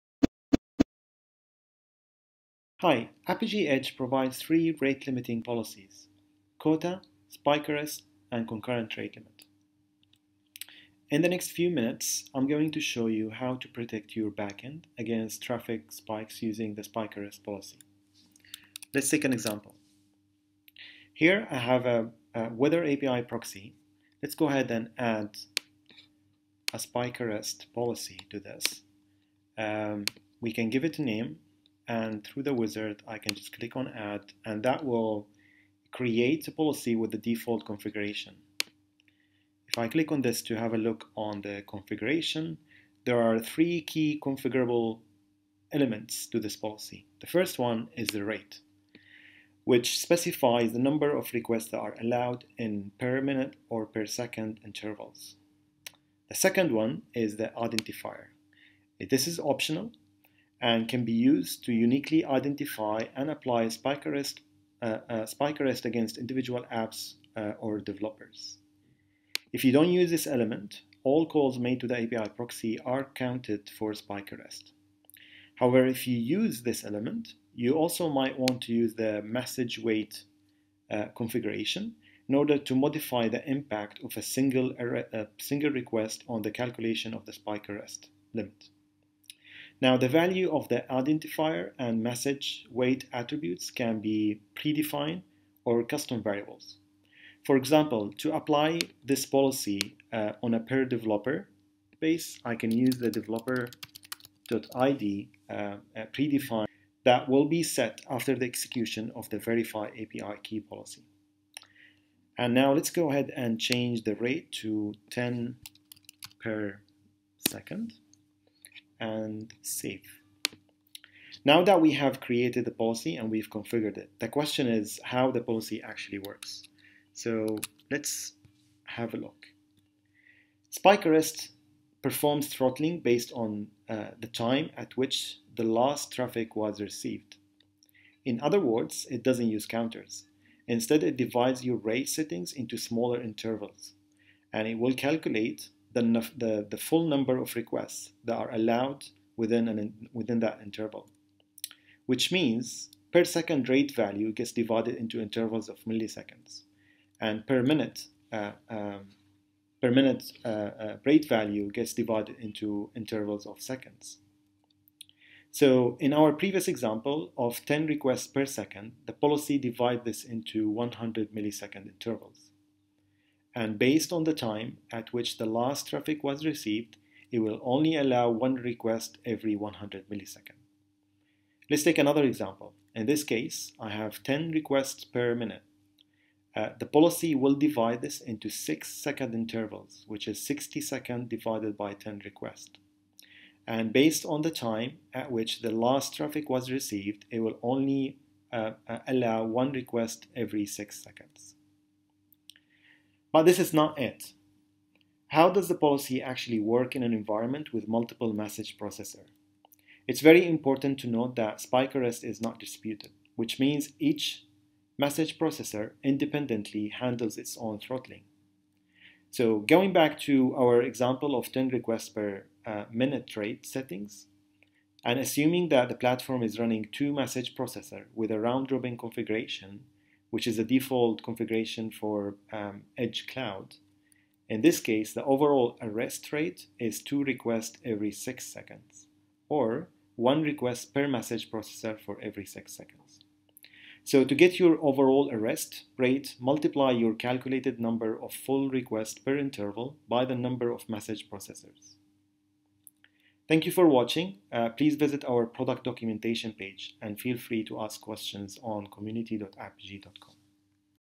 Spike Arrest
Protect your API proxy's target backend against severe traffic spikes and denial of service attacks by implementing the Spike Arrest Policy. Implement Apigee's out of the box Spike Arrest policy in under 3 minutes to ensure that the exposure of your infrastructure is in line with its capacity.